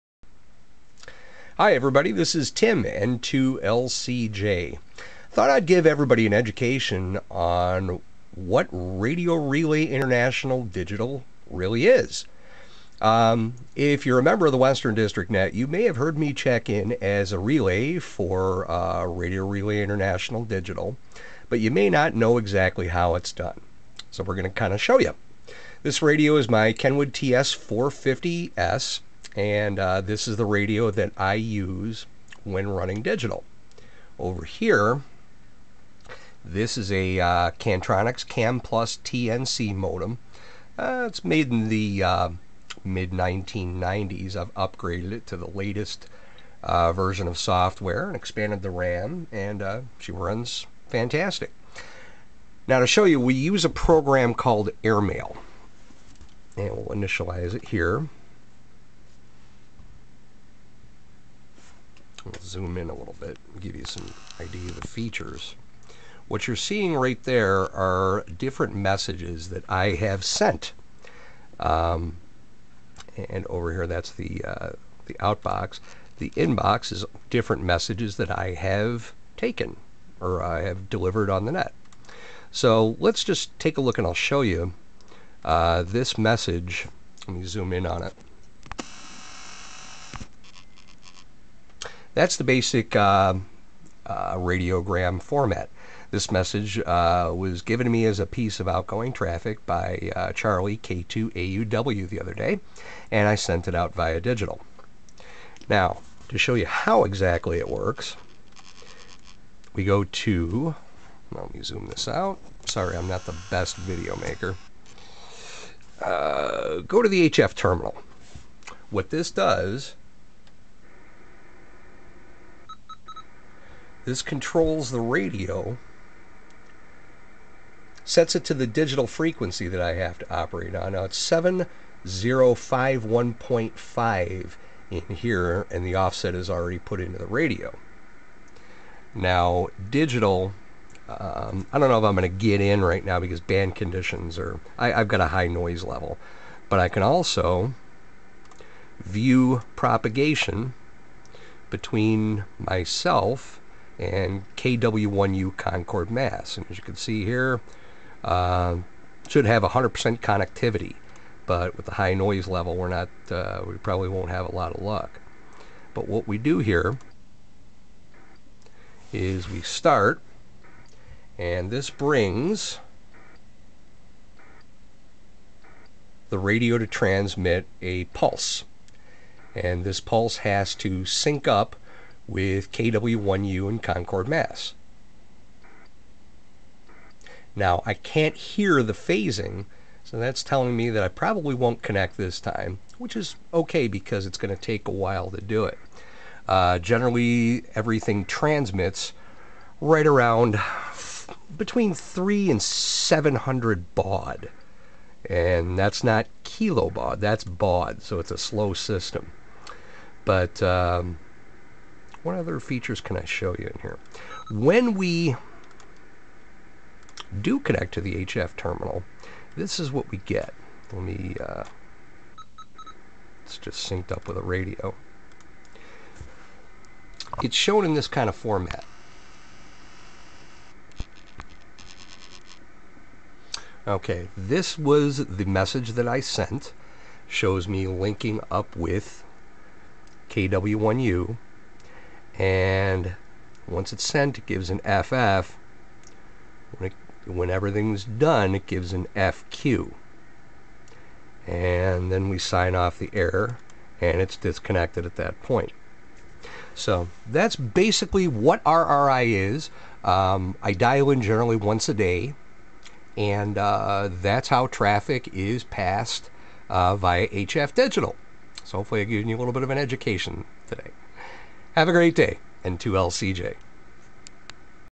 Introduction to operating a Radio Relay International station
This video shows how Radiogram messages are sent and received via Radio Relay International using a digital relay and an over-the-air modem